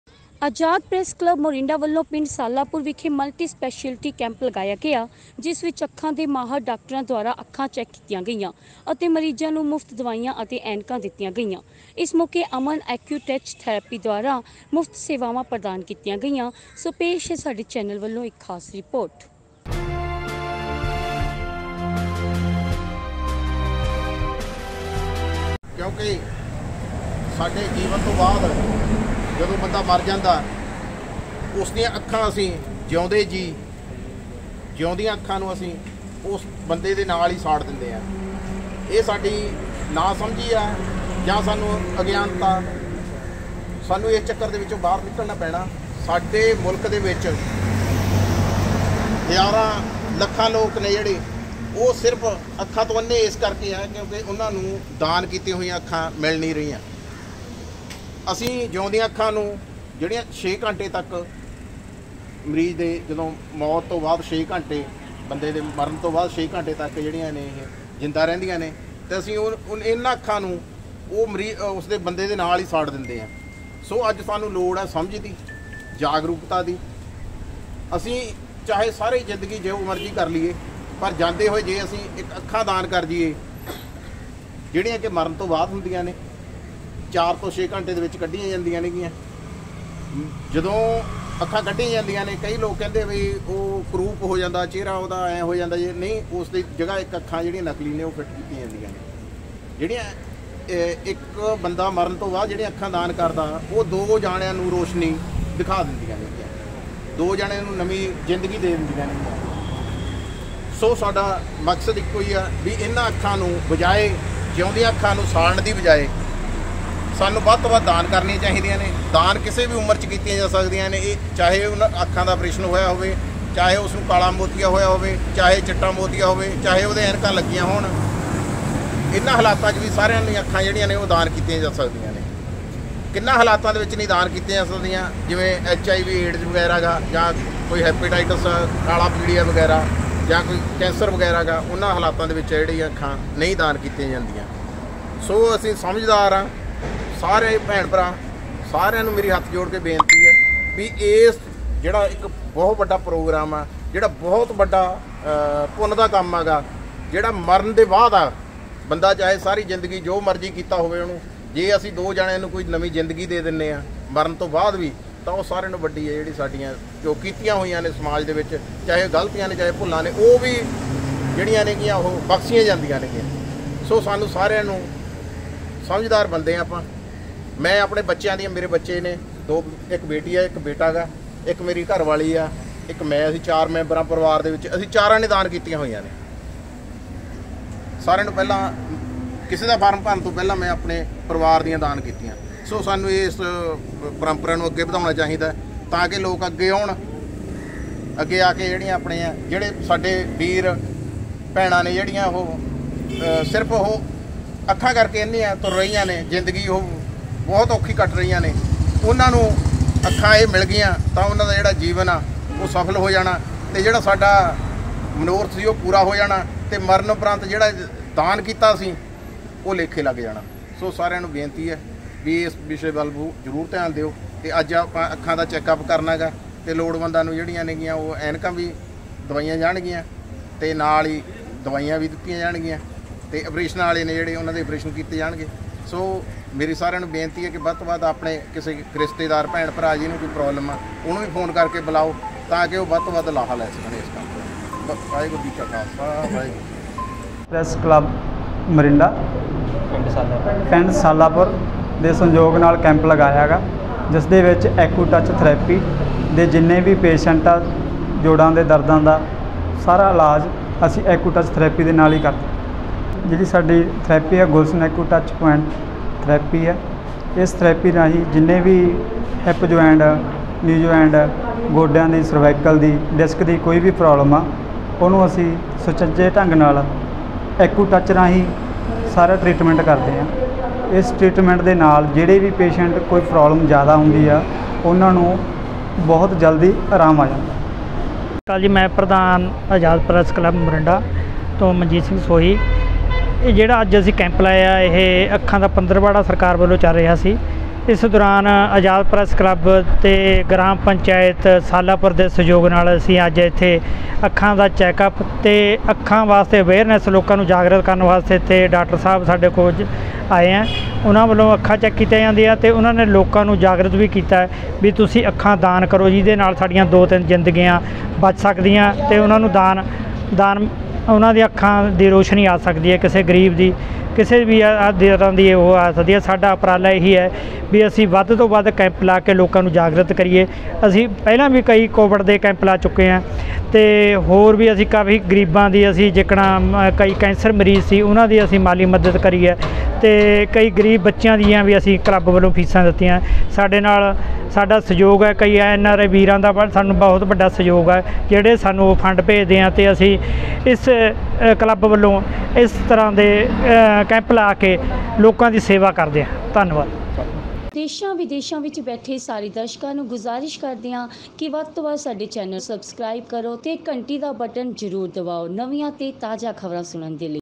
ਸਮਾਜ ਸੇਵਾ ਵਿੱਚ ਮੋਹਰੀ ਹੈ , ਅਜ਼ਾਦ ਪ੍ਰੈਸ ਕਲੱਬ ਮੋਰਿੰਡਾ
ਸਮਾਜ ਸੇਵਾ ਵਿੱਚ ਮੋਹਰੀ ਹੈ ਅਜ਼ਾਦ ਪ੍ਰੈਸ ਕਲੱਬ ਮੋਰਿੰਡਾ | ਪ੍ਰੈਸ ਕਲੱਬ ਵੱਲੋਂ ਲਗਵਾਇਆ ਗਿਆ ਅੱਖਾਂ ਦਾ ਕੈੰਪ